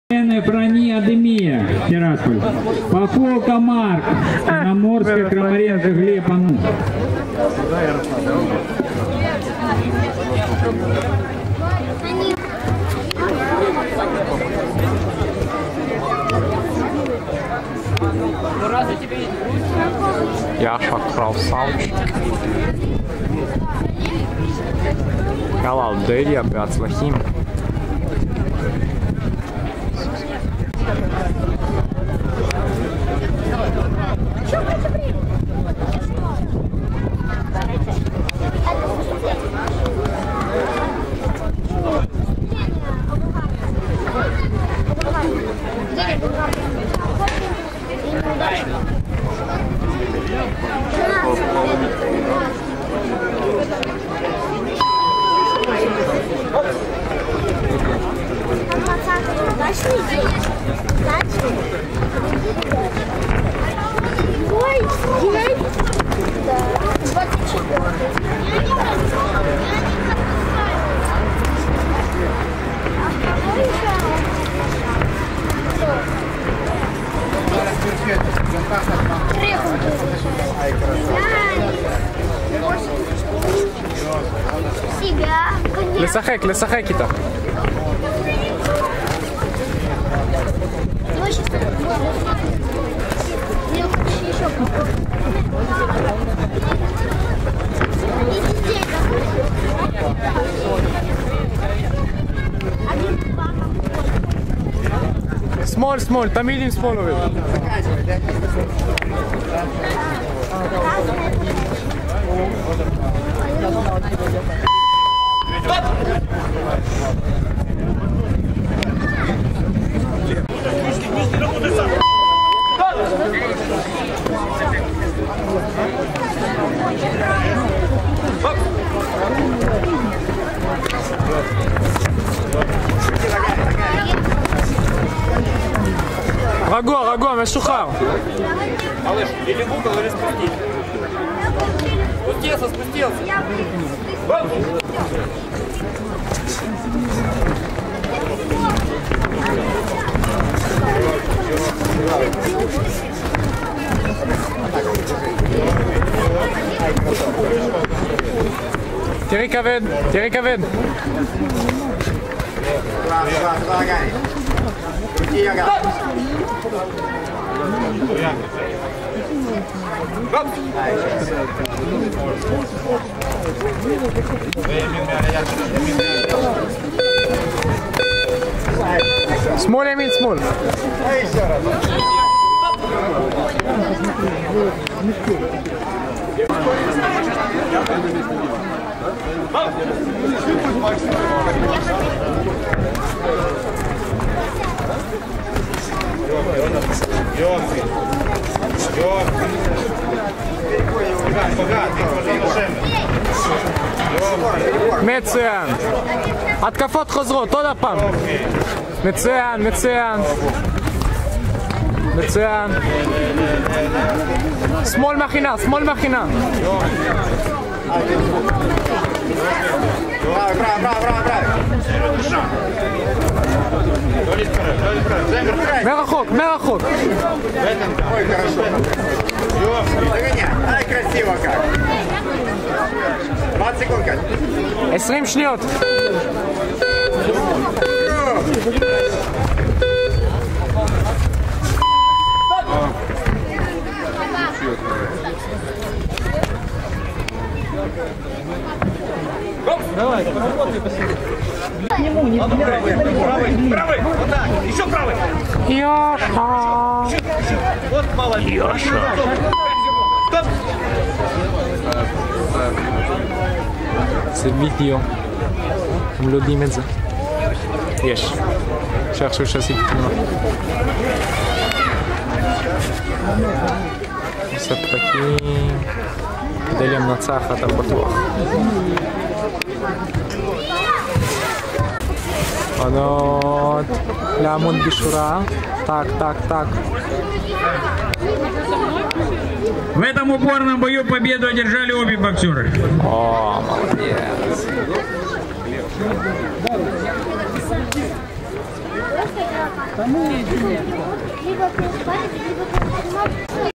אודליה - ספרינג במחנה אימונים במולדובה 13.07.2018
מחנה אימוני איגרוף במולדובה